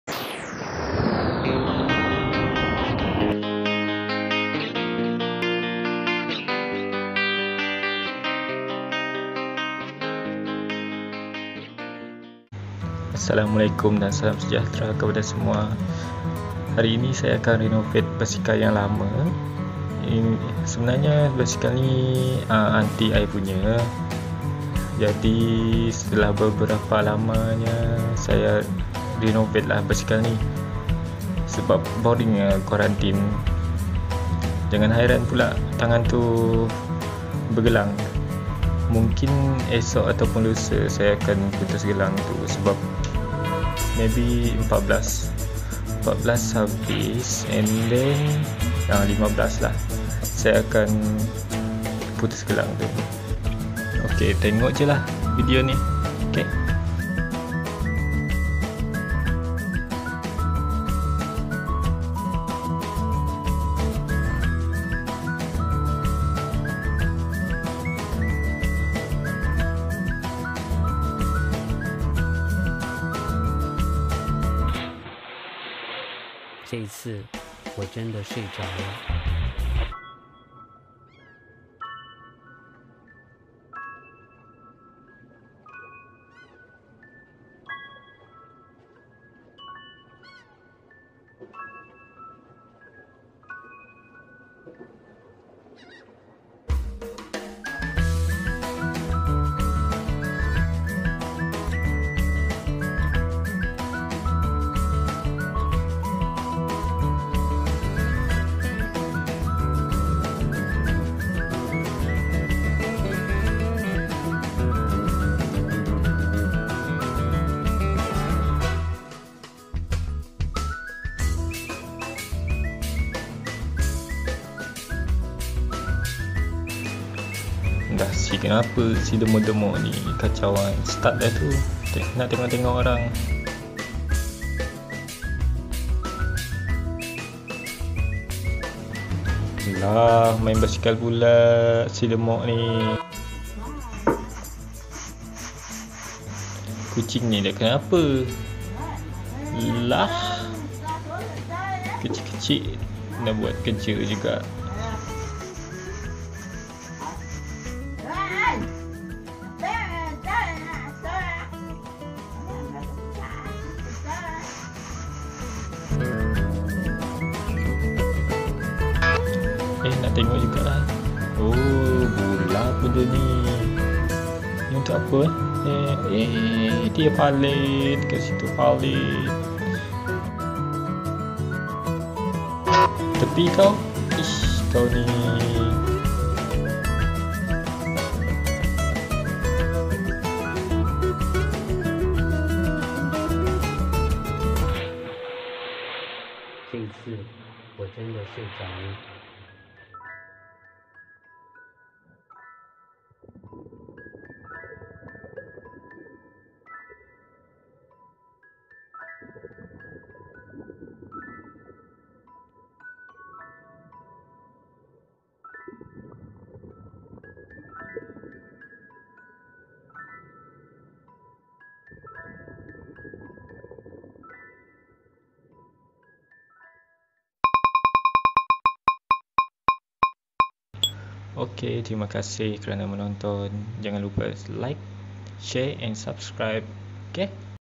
Renew Basikal Terpaling Buruk?
Sekian lama terperap dalam rumah kerana kuarantin 10 hari tiba-tiba rajin merasuki diri untuk renew basikal yang bertahun diperap sampai berkarat beb.